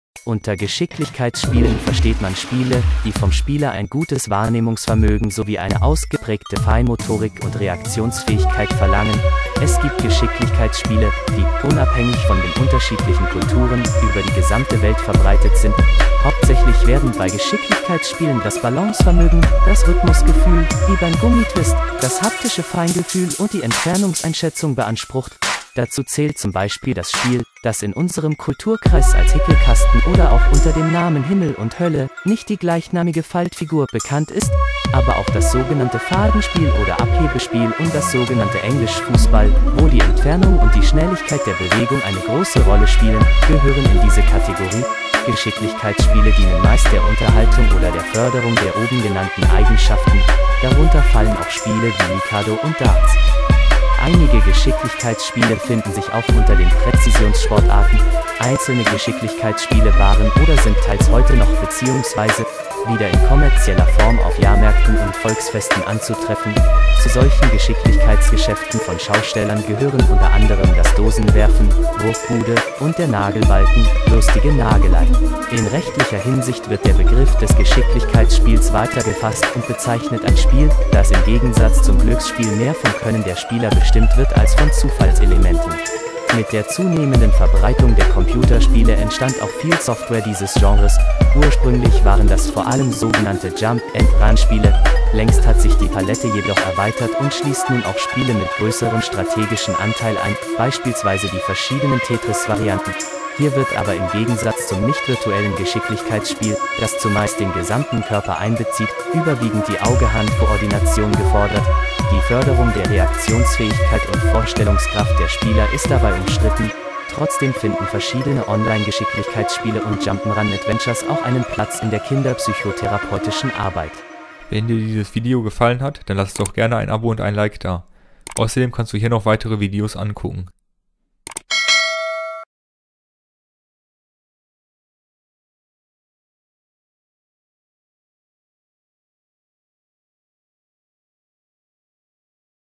Geschicklichkeitsspiele
Unter Geschicklichkeitsspielen versteht man Spiele, die vom Spieler ein gutes Wahrnehmungsvermögen sowie eine ausgeprägte Feinmotorik und Reaktionsfähigkeit verlangen. Es gibt Geschicklichkeitsspiele, die, unabhängig von den unterschiedlichen Kulturen, über die gesamte Welt verbreitet sind. Hauptsächlich werden bei Geschicklichkeitsspielen das Balancevermögen, das Rhythmusgefühl (wie beim Gummitwist), das haptische Feingefühl und die Entfernungseinschätzung beansprucht. Dazu zählt zum Beispiel das Spiel, das in unserem Kulturkreis als Hickelkasten oder auch unter dem Namen Himmel und Hölle (nicht die gleichnamige Faltfigur) bekannt ist. Aber auch das sogenannte Fadenspiel oder Abhebespiel und das sogenannte Englisch Fußball, wo die Entfernung und die Schnelligkeit der Bewegung eine große Rolle spielen, gehören in diese Kategorie. Geschicklichkeitsspiele dienen meist der Unterhaltung oder der Förderung der oben genannten Eigenschaften. Darunter fallen auch Spiele wie Mikado und Darts.

Einige Geschicklichkeitsspiele finden sich auch unter den Präzisionssportarten. Einzelne Geschicklichkeitsspiele waren oder sind teils heute noch bzw. wieder in kommerzieller Form auf Jahrmärkten und Volksfesten anzutreffen. Zu solchen Geschicklichkeitsgeschäften von Schaustellern gehören unter anderem das Dosenwerfen („Wurfbude“) und der Nagelbalken („Lustige Nagelei“).
In rechtlicher Hinsicht wird der Begriff des Geschicklichkeitsspiels weiter gefasst und bezeichnet ein Spiel, das im Gegensatz zum Glücksspiel mehr vom Können der Spieler bestimmt wird als von Zufallselementen.

Mit der zunehmenden Verbreitung der Computerspiele entstand auch viel Software dieses Genres. Ursprünglich waren das vor allem sogenannte Jump ’n’ Run-Spiele. Längst hat sich die Palette jedoch erweitert und schließt nun auch Spiele mit größerem strategischen Anteil ein, beispielsweise die verschiedenen Tetris-Varianten. Hier wird aber im Gegensatz zum nicht virtuellen Geschicklichkeitsspiel, das zumeist den gesamten Körper einbezieht, überwiegend die Auge-Hand-Koordination gefordert. Die Förderung der Reaktionsfähigkeit und Vorstellungskraft der Spieler ist dabei umstritten. Trotzdem finden verschiedene Online-Geschicklichkeitsspiele und Jump’n’Run-Adventures auch einen Platz in der kinderpsychotherapeutischen Arbeit.